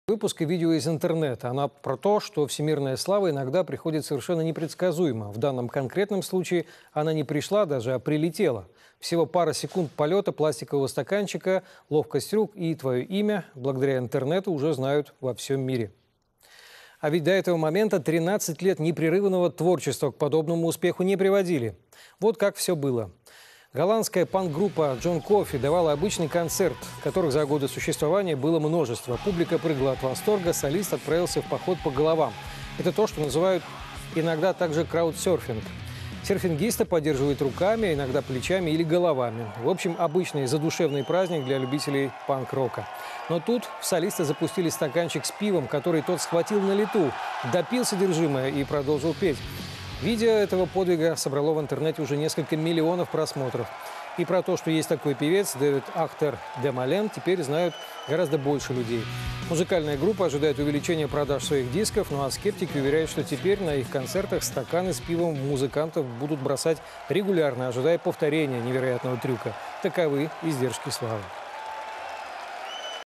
Панк ловко паймал стакан с пивом / John Coffey Awesome Beer Catch
Лидер голландской рок-группы John Coffey Дэвид Ахтер де Молен поразил своей невероятной ловкостью.
Во время выступления на фестивале под открытым небом Pinkpop он умудрился поймать брошенный одним из фанатов пластиковый стакан с пивом. Ситуация тем более удивительна, что при этом музыкант стоял на руках и плечах у зрителей.
Ловко схватив летевший в его сторону стакан, певец выпил пиво и как ни в чем не бывало выбросил тару.